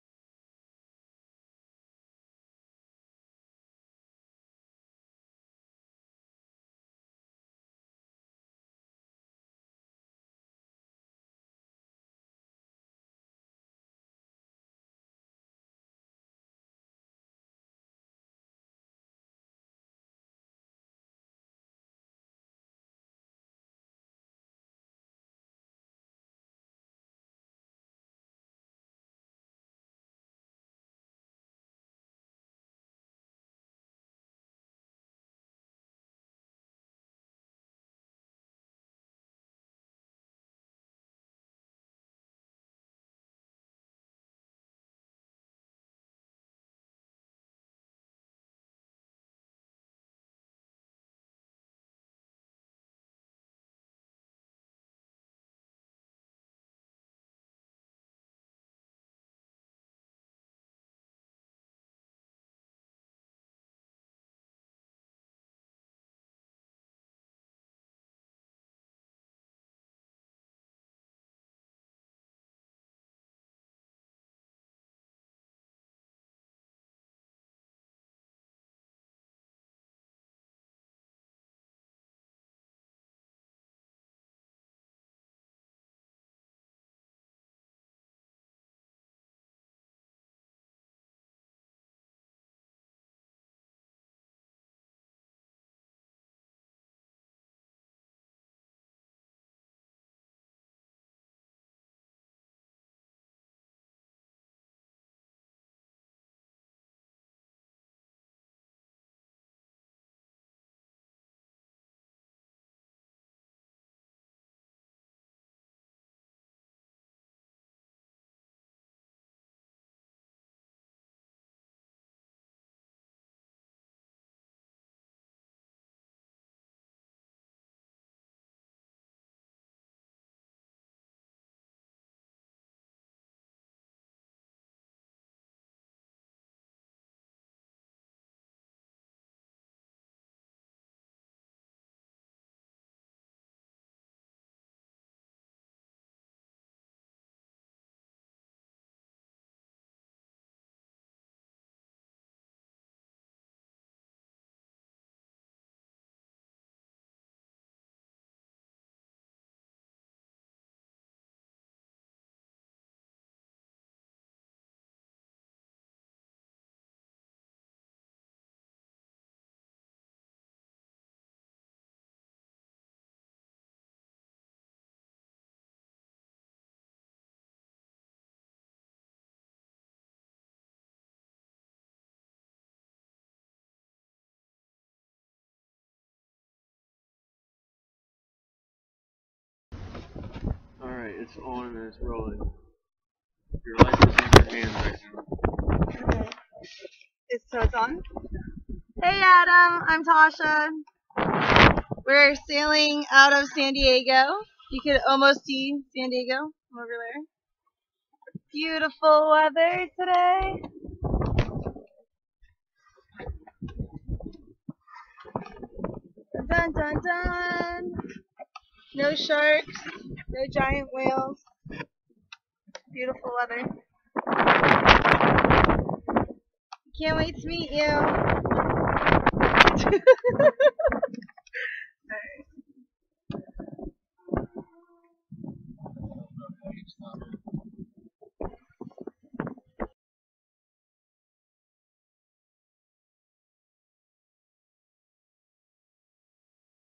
$500 Cheap Sailboat - Unbelievable steal
Right place at the right time. Scored my new boat for a crazy $500!!

I had many memories on this boat for the short time i had it.. I should have kept it knowing what i do now.. it was a rock solid TANK..